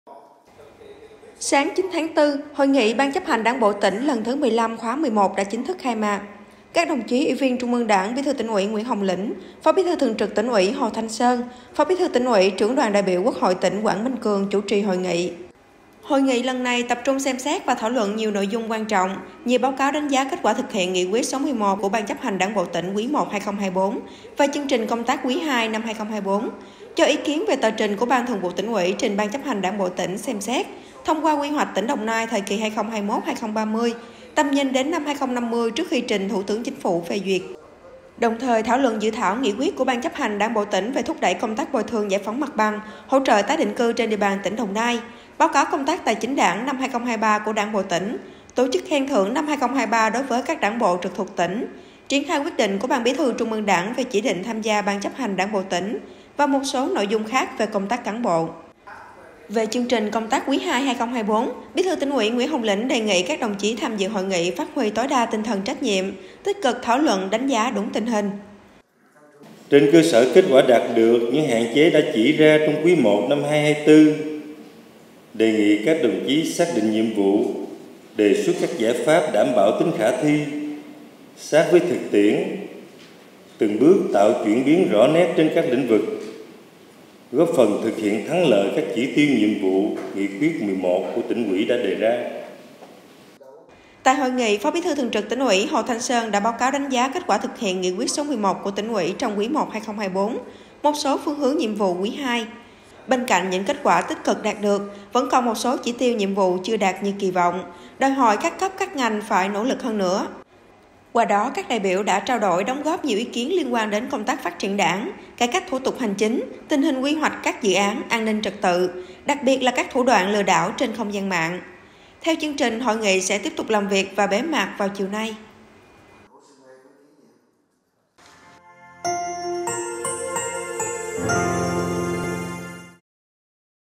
Khai mạc Hội nghị Ban Chấp hành Đảng bộ tỉnh lần thứ 15 khóa XI
Sáng 9-4, Hội nghị Ban Chấp hành Đảng bộ tỉnh lần thứ 15 (khóa XI) đã chính thức khai mạc. Các đồng chí: Ủy viên Trung ương Đảng, Bí thư Tỉnh ủy Nguyễn Hồng Lĩnh; Phó bí thư thường trực Tỉnh ủy Hồ Thanh Sơn; Phó bí thư Tỉnh ủy, Trưởng đoàn Đại biểu Quốc hội Quản Minh Cường chủ trì hội nghị.